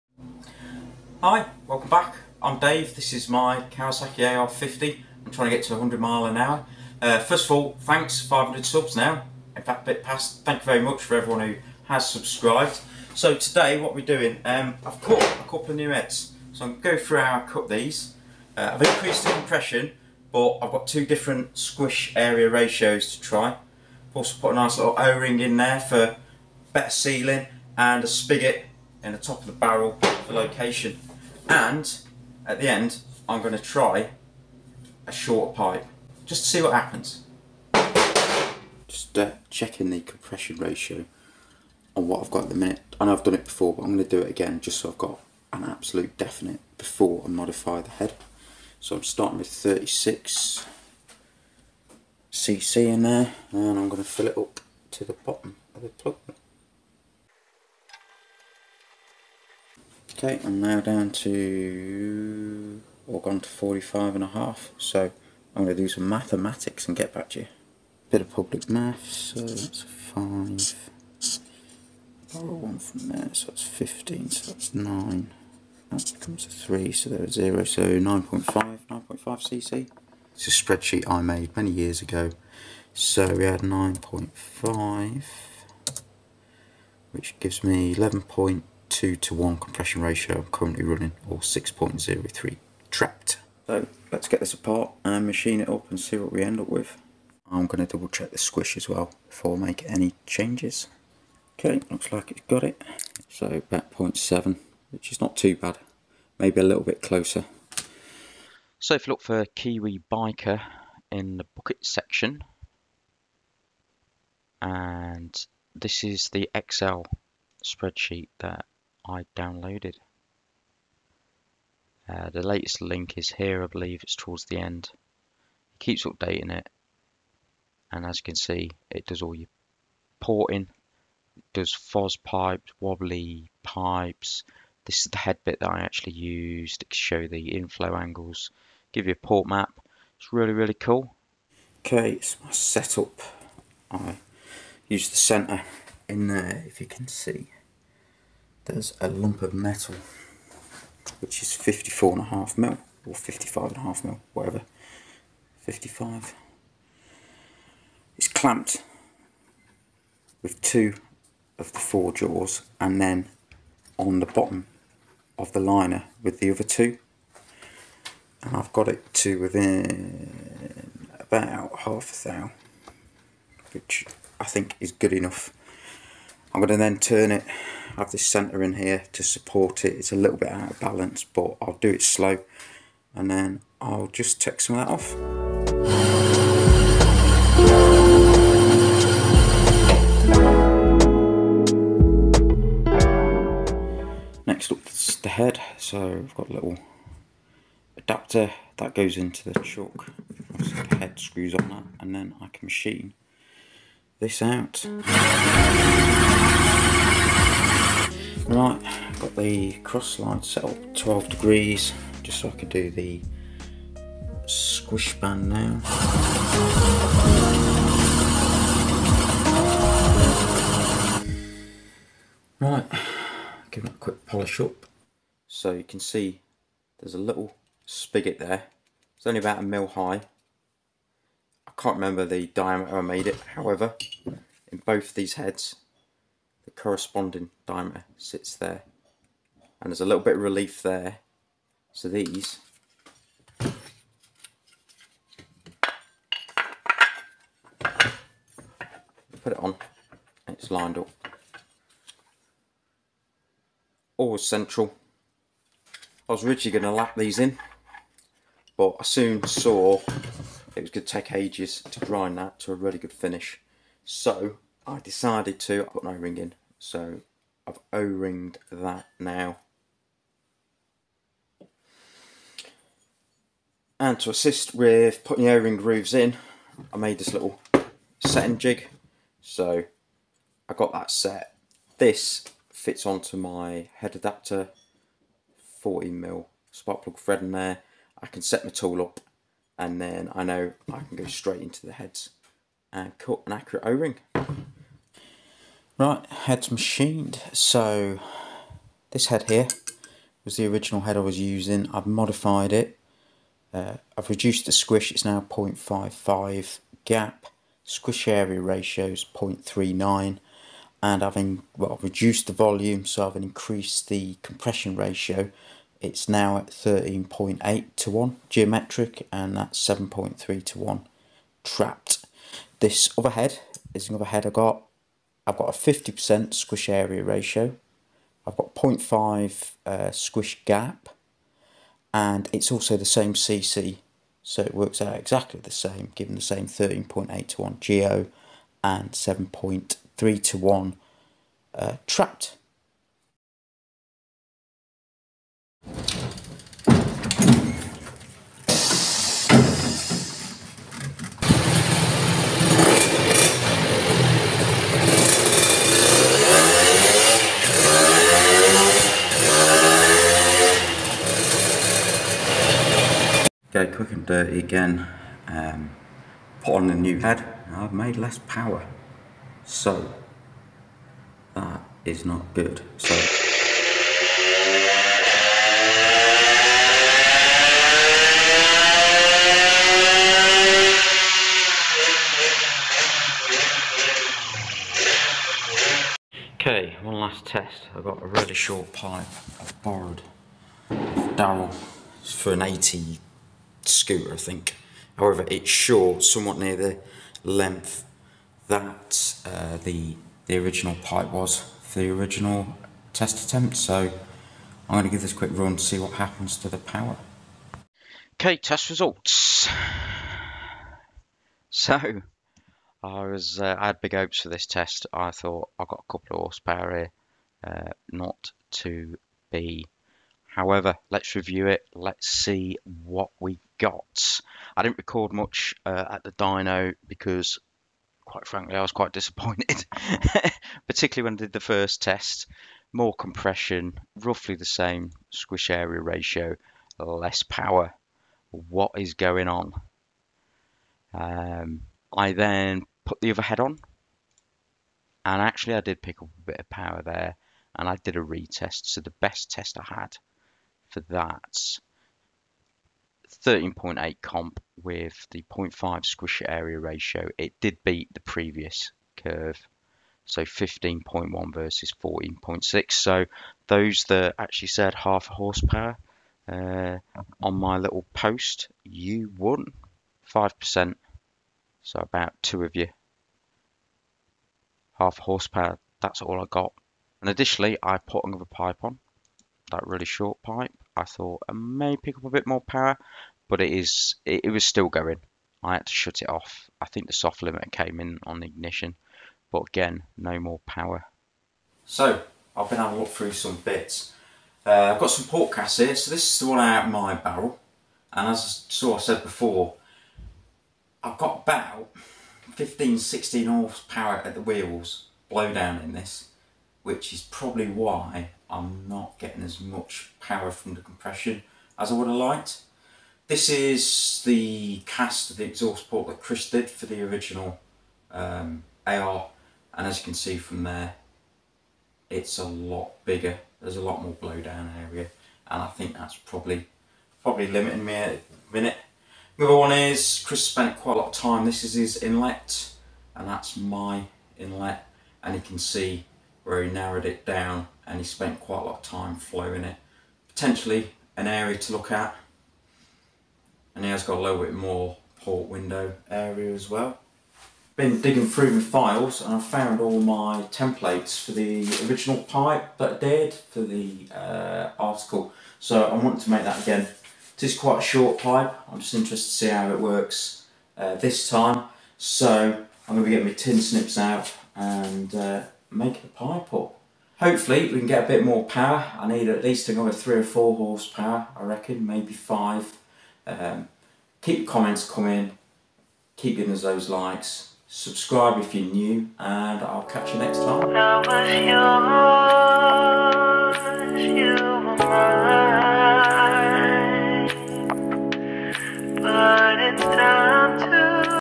2 Stroke Squish Area Ratio Test - Plus Bonus PIPE test
Well... I thought I'd try some more compression for an easy power increase... 2 strokes!